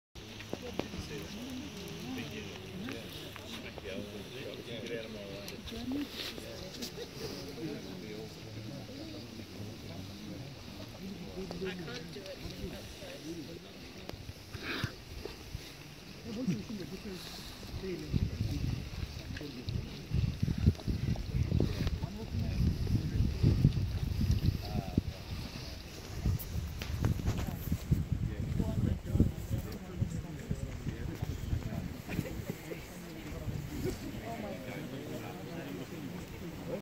Tasmanian Unzoo - Kangaroo Area
Was feeding the Kangaroo, new experiences and love it!